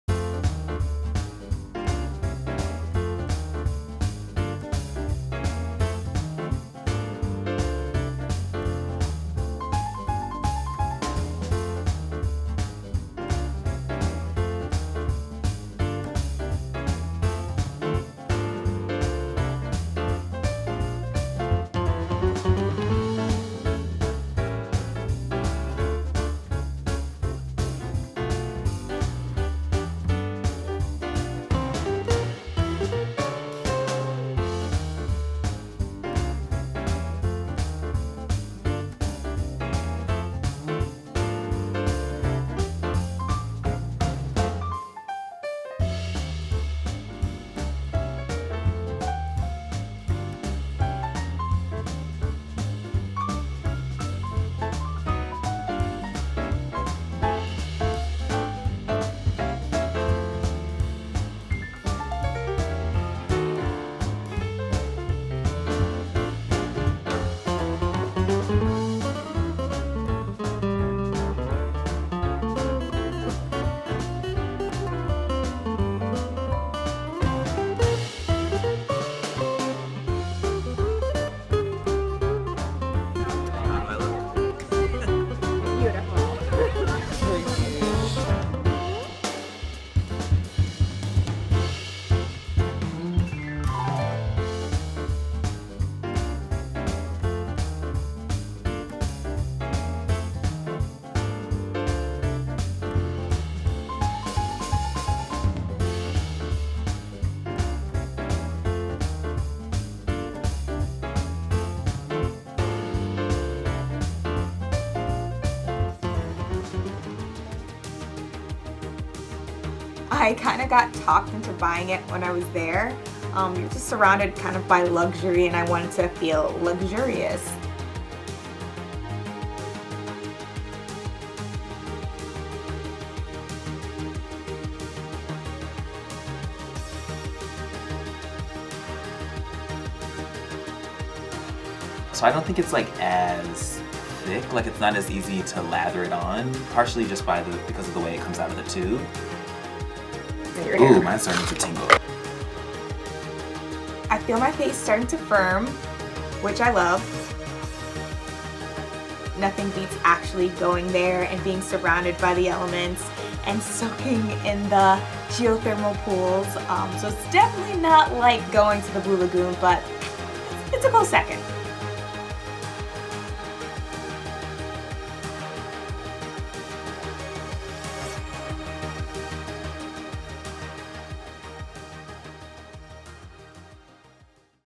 Iceland's Blue Lagoon Beauty Routine
Iceland's Blue Lagoon is one of the country's biggest tourist destinations. It's also the perfect place to get beautiful skin with its three-step regimen: a lava scrub, silicia mud mask, and algae mask. If you'll miss the glow up process, you can always bring the beauty products home! We tried the take-home mud masks to see if it's worth taking a bit of Iceland home.

So, is a trip to the Blue Lagoon is really worth it?